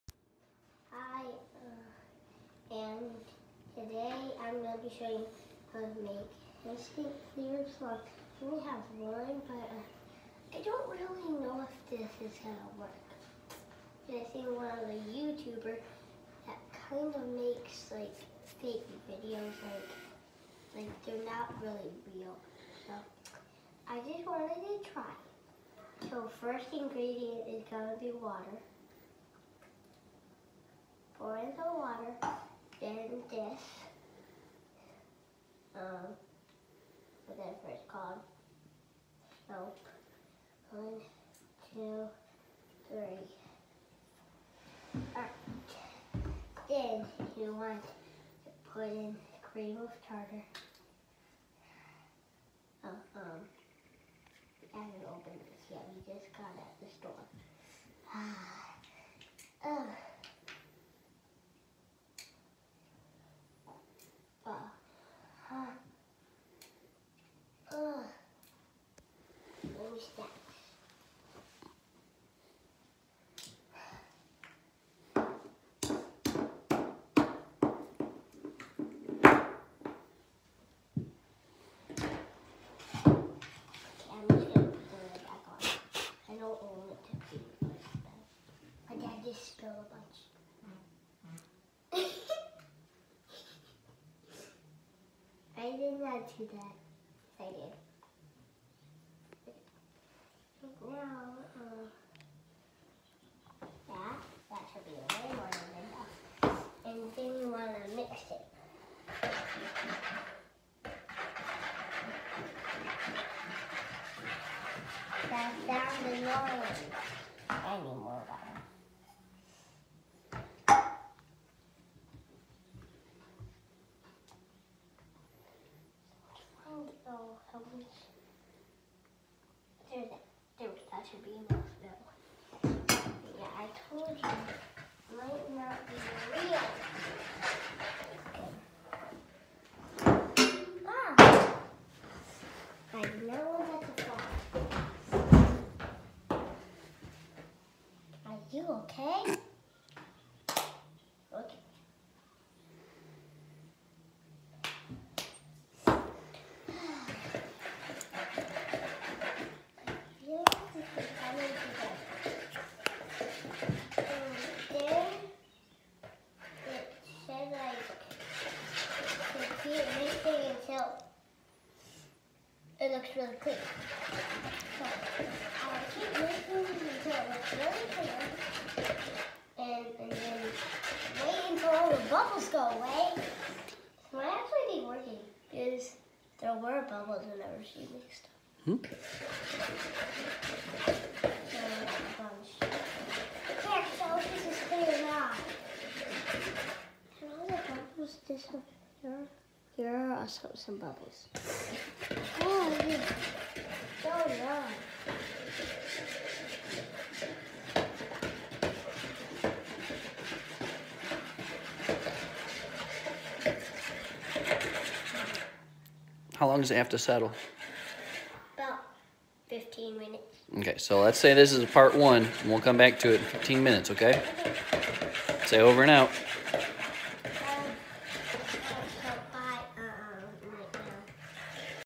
Awesome Slime 3 part 1
Trying clear slime with the cream of tartar we just got at the store.